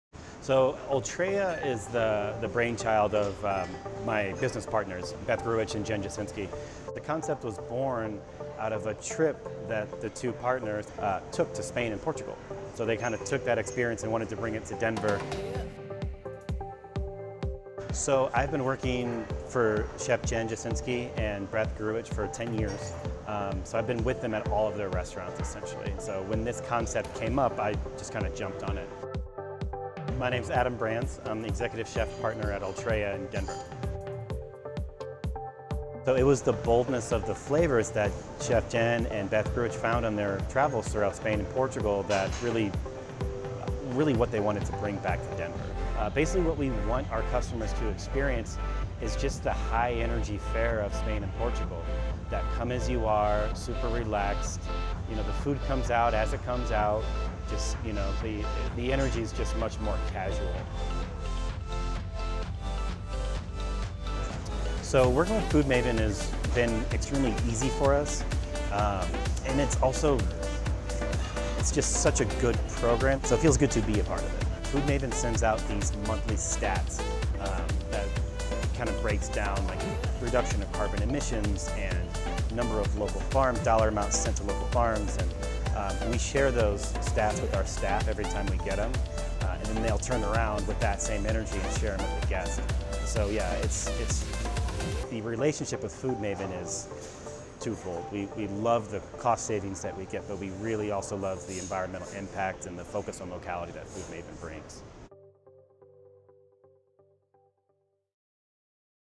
Ultreia: Spanish Flavors with a Sustainable Twist
Located in Union Station in Denver, Ultreia is the result of a trip to Spain from co-owners Jennifer Jasinski and Beth Gruitch. The menu is based on the shared plate pintxos (pronounced peen chos) and tapas of the Iberian peninsula countries of Spain and Portugal.

FoodMaven helps Executive Chef Adam source local, sustainable food and source ingredients for a menu that is diverse, incredibly favorable and reminiscent of Spain.

The word Ultreia has its roots in Latin, loosely translates to “onward” and refers to the words of encouragement shouted to pilgrims on their Camino de Santiago – a 1,000-year-old pilgrimage to the shrine of St. James in the Cathedral of Santiago de Compostela in Spain.